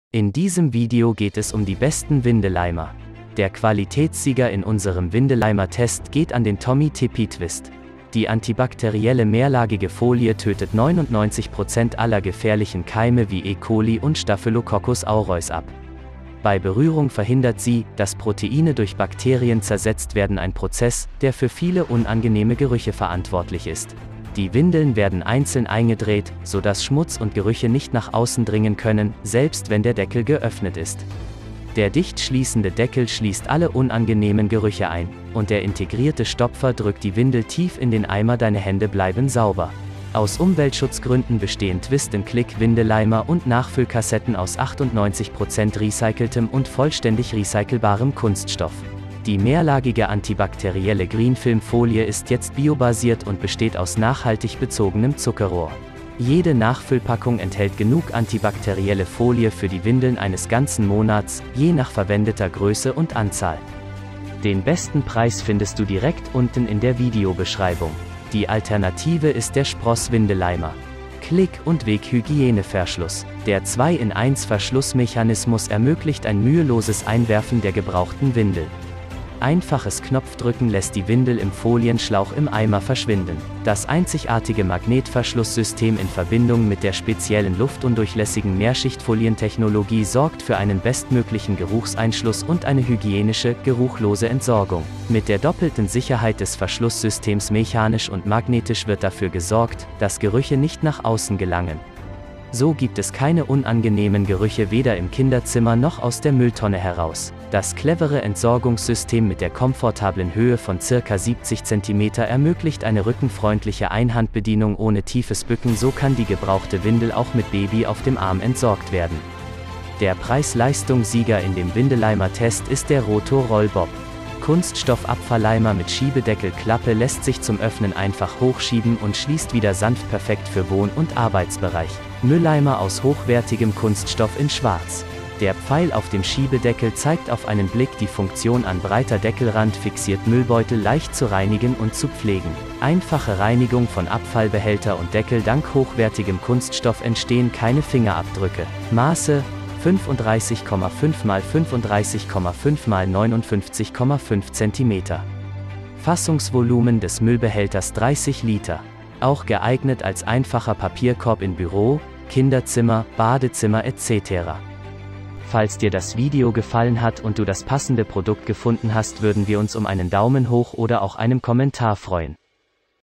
ᐅ Windeleimer Test 2025 | Beste Windeleimer vorgestellt
in diesem Video geht es um die besten Windeleimer.

Der Qualitätssieger in unserem Windeleimer Test geht an den Tommee Tipee Twist.

Die antibakterielle mehrlagige Folie tötet 99 Prozent aller gefährlichen Keime wie E coli und Staphylococcus aureus ab. Bei Berührung verhindert sie, dass Proteine durch Bakterien zersetzt werden ein Prozess, der für viele unangenehme Gerüche verantwortlich ist.

Die Windeln werden einzeln eingedreht, sodass Schmutz und Gerüche nicht nach außen dringen können, selbst wenn der Deckel geöffnet ist. Der dicht schließende Deckel schließt alle unangenehmen Gerüche ein, und der integrierte Stopfer drückt die Windel tief in den Eimer deine Hände bleiben sauber.

Aus Umweltschutzgründen bestehen Twist and Click Windeleimer und Nachfüllkassetten aus 98 Prozent recyceltem und vollständig recycelbarem Kunststoff.

Die mehrlagige antibakterielle Greenfilm Folie ist jetzt biobasiert und besteht aus nachhaltig bezogenem Zuckerrohr.

Jede Nachfüllpackung enthält genug antibakterielle Folie für die Windeln eines ganzen Monats, je nach verwendeter Größe und Anzahl.

Die alternative ist der Spross Windeleimer.

Klick und Weg Hygieneverschluss. Der 2 in eins Verschlussmechanismus ermöglicht ein müheloses Einwerfen der gebrauchten Windel. Einfaches Knopfdrücken lässt die Windel im Folienschlauch im Eimer verschwinden.
Das einzigartige Magnetverschlusssystem in Verbindung mit der speziellen luftundurchlässigen Mehrschichtfolientechnologie sorgt für einen bestmöglichen Geruchseinschluss und eine hygienische, geruchlose Entsorgung.
Mit der doppelten Sicherheit des Verschlusssystems Mechanisch und Magnetisch wird dafür gesorgt, dass Gerüche nicht nach außen gelangen. So gibt es keine unangenehmen Gerüche weder im Kinderzimmer noch aus der Mülltonne heraus.
Das clevere Entsorgungssystem mit der komfortablen Höhe von ca 70 Zentimeter ermöglicht eine rückenfreundliche Einhandbedienung ohne tiefes Bücken so kann die gebrauchte Windel auch mit Baby auf dem Arm entsorgt werden.
Der Preis Leistung Sieger in dem Windeleimer Test ist der Rotho Roll Bob.

Kunststoff Abfalleimer mit Schiebedeckel Klappe lässt sich zum Öffnen einfach hochschieben und schließt wieder sanft perfekt für Wohn und Arbeitsbereich.
Mülleimer aus hochwertigem Kunststoff in schwarz. Der Pfeil auf dem Schiebedeckel zeigt auf einen Blick die Funktion an breiter Deckelrand fixiert Müllbeutel Leicht zu reinigen und zu pflegen.
Einfache Reinigung von Abfallbehälter und Deckel dank hochwertigem Kunststoff entstehen keine Fingerabdrücke.
Maße: 35,5 mal 35,5 mal 59,5 Zentimeter. Fassungsvolumen des Müllbehälters 30 Liter. Auch geeignet als einfacher Papierkorb in Büro, Kinderzimmer, Badezimmer etcetera.